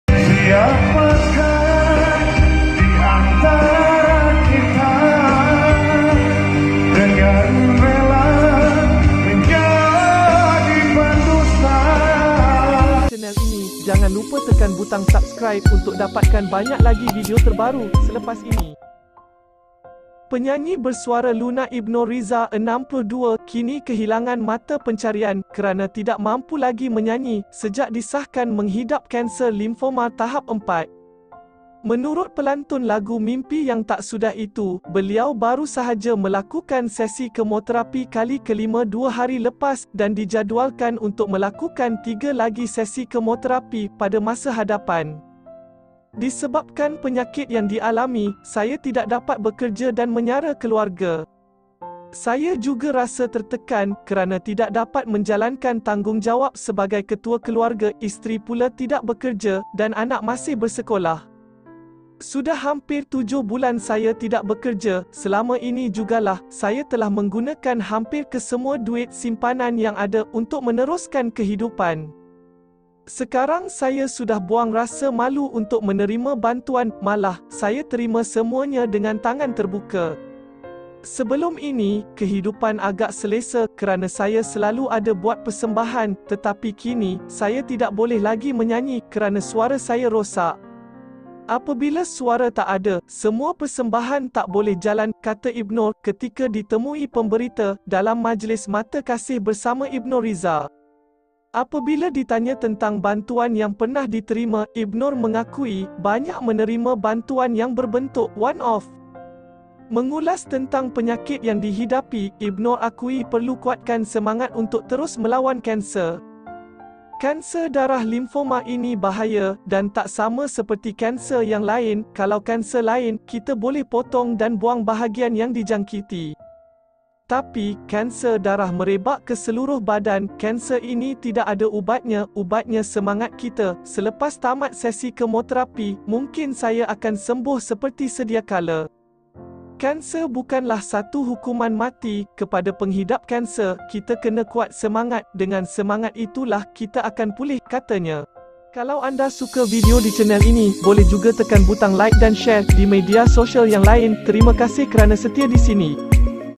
TAK SANGKA,Begini KEHIDUPAN Penyanyi IBNOR RIZA Kerana Penyakit KANSER."Saya Rasa TERTEKAN Kerana.."
📵 Editing : Samsung Note 20 Ultra